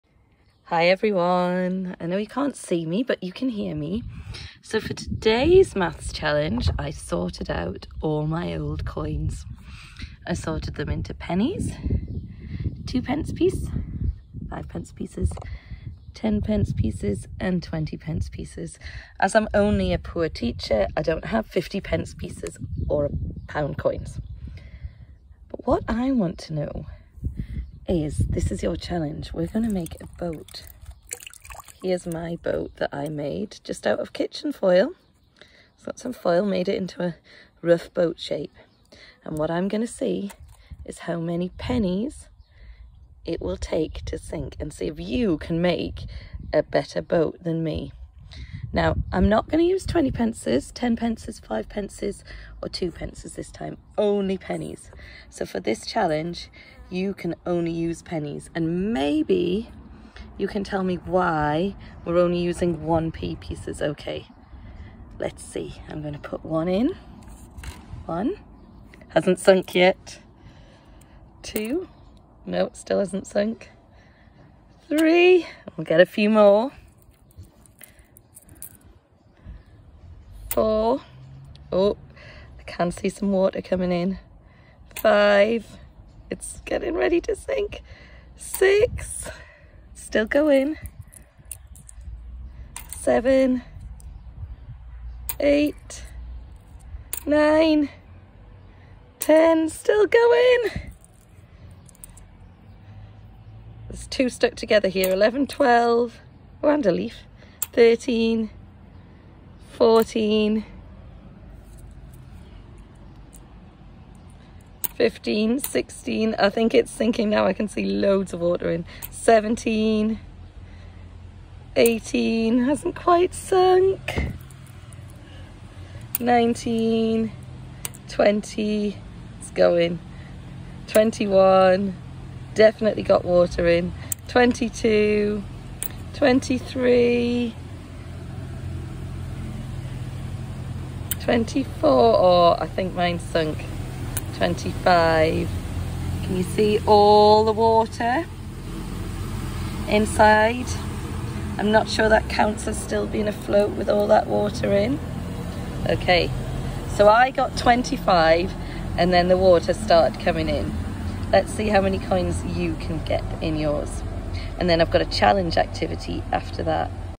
Boat maths challenge
Can you make a better boat than me? See how many 1p pieces it takes to sink your boat?